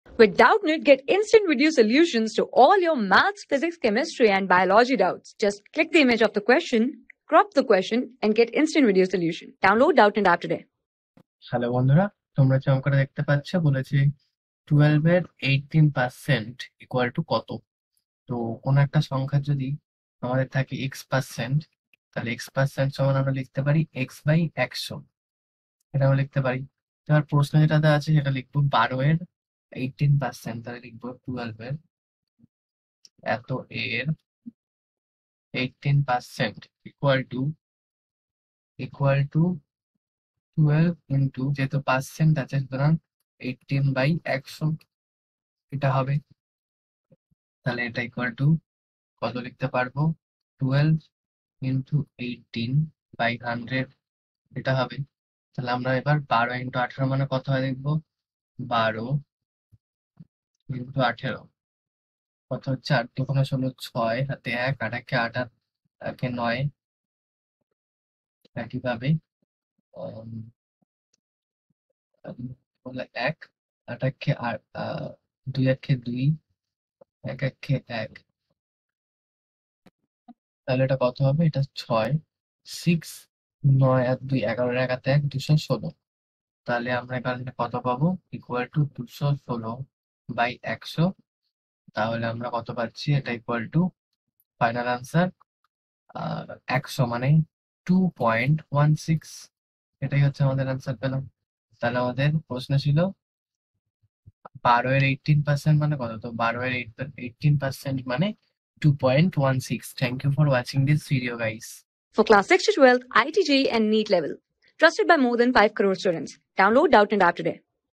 The mean square deviation of a set of m observationsy_1, y_2…..y_mabout a point K is defined as1...
The mean square deviation of a set of m observationsy_1, y_2…..y_mabout a point K is defined as1/m sum_(i = 1)^(m) (y_i - k)^(2). The mean square deviation about-3 and 3 are 16 and 8 respectively, then standard deviation of this set of observation?
Class: 12
Subject: MATHS
Chapter: MOCK TEST 2
Board:IIT JEE

👉 Have Any Query? Ask Us.